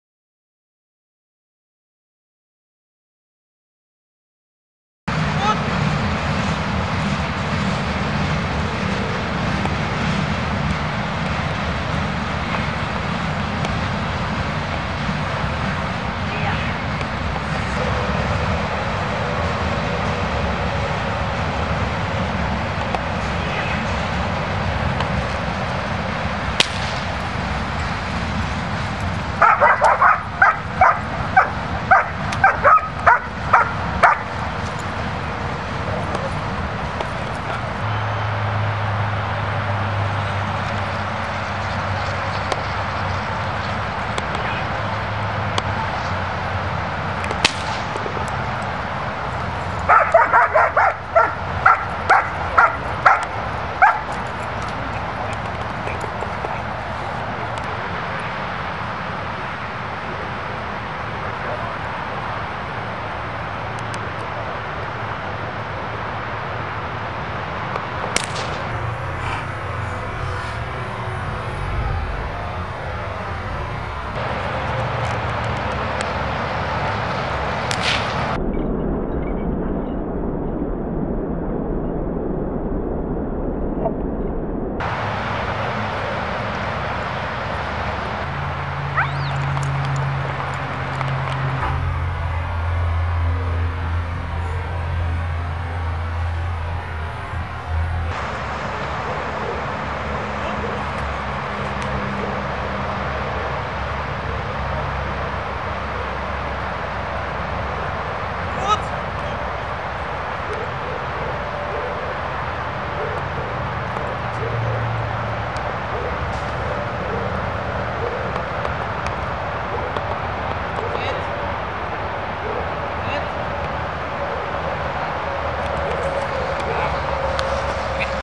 Ha's Lilo
H. Phams awesome gsd. Lilo v Heidhof training a schutzhund one routine.